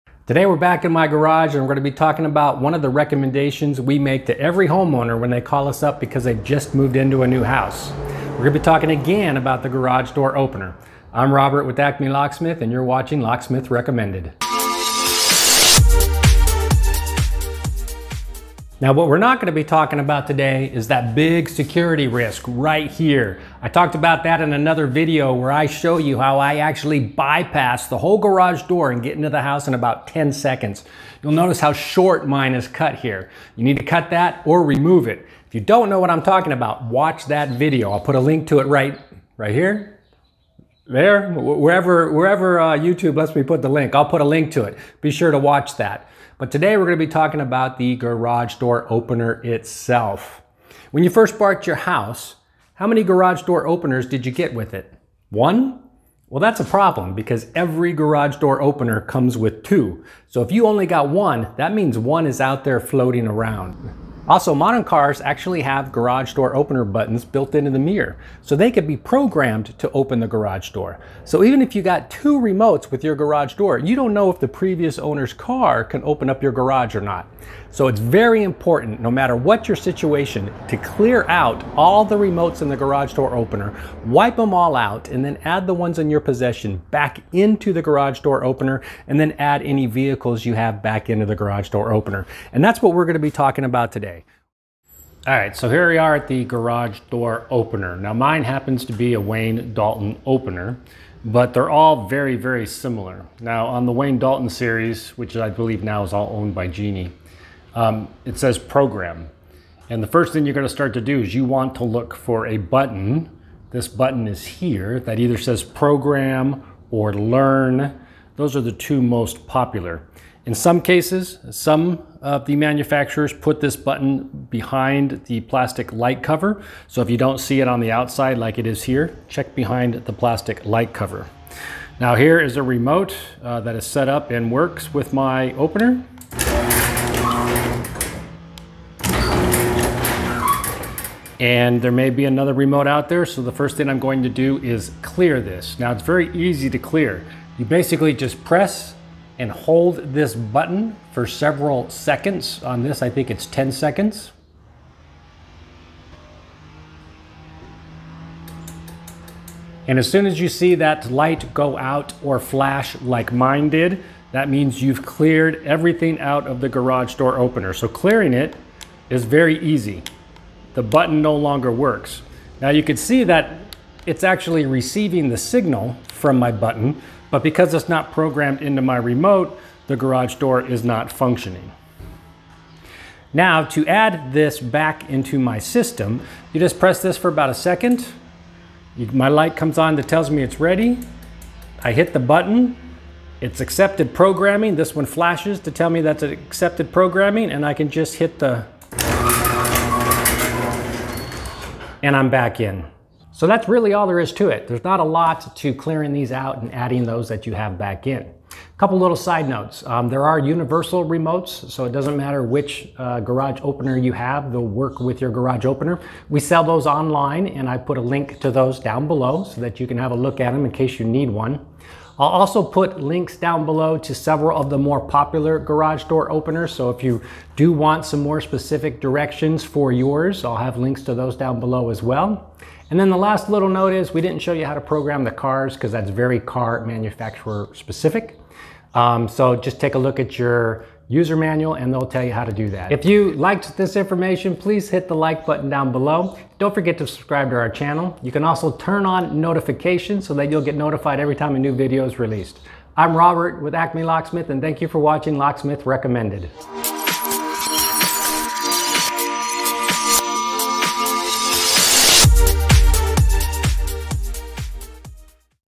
How to Reset a Garage Door Opener and Add Remotes
When you've just bought a home, this is one of the 7 Security Items that should to be addressed as soon as you've moved in: Erase the Garage Door Opener Memory and reset the garage door opener so that all of the old garage remotes and cars programmed to open it will no longer open the garage.

Clearing the garage door opener will ensure that any garage door remote or car that was programmed to open the garage, not in your possession, will no longer open your garage door. Reprogramming a garage door opener is something that is easy and simple enough so that every new home owner can do so on their own.

See me Break into a Home in About 10 Seconds Using the Garage Door Opener's Emergency Release: